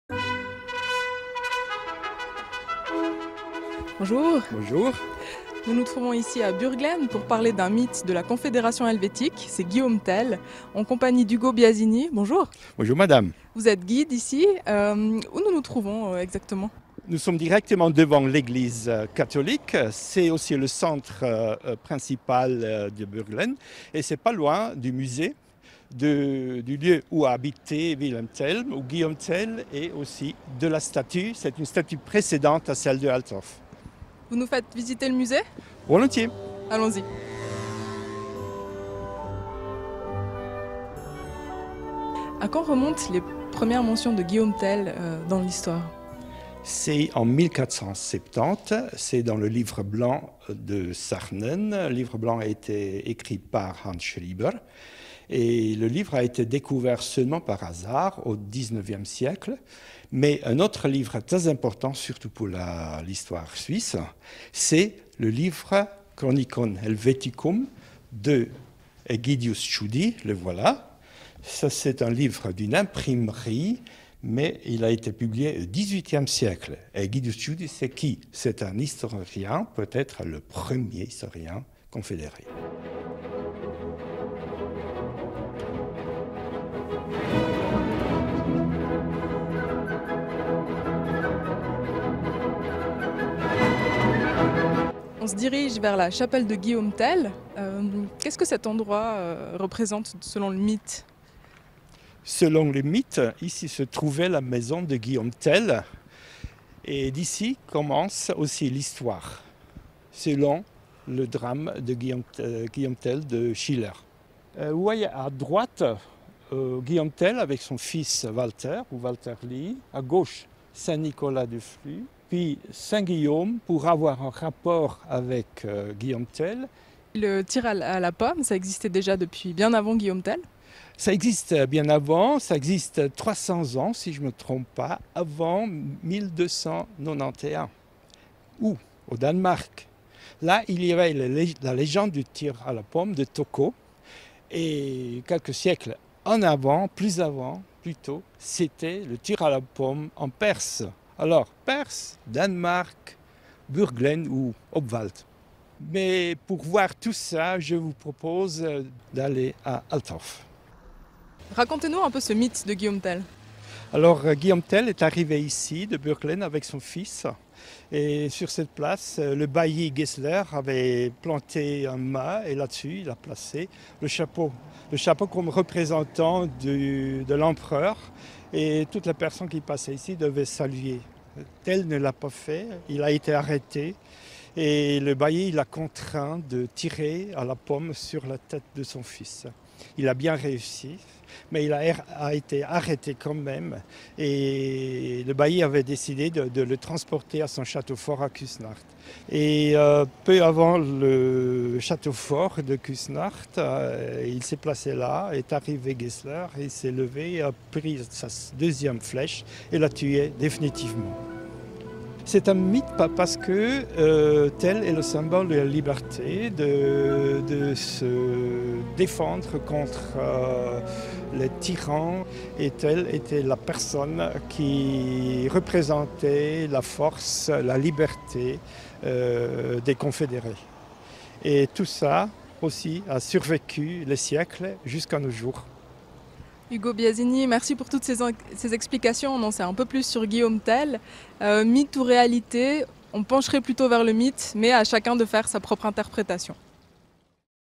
Guillaume Tell: sur les traces d’une légende suisse
Canal9 est parti sur les traces d’une légende érigée en héros du roman national suisse à Bürglen (UR), en compagnie d’Hugo Biasini, guide au musée Guillaume Tell.